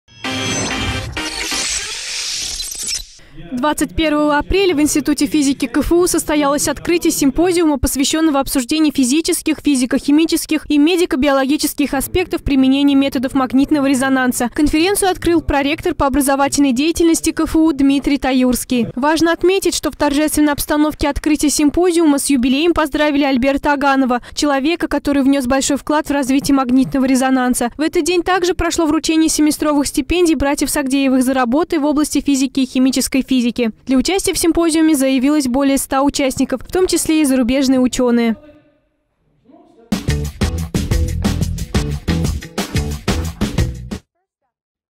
РЕЗОНАНС от 21.04.2016
Нужно знать, что такое важное открытие как магнитный резонанс открыли именно в Казанском университете. Так вот с 21 по 23 апреля в Институте физики КФУ проходит симпозиум, посвященный обсуждению вопросов по применению методов магнитного резонанса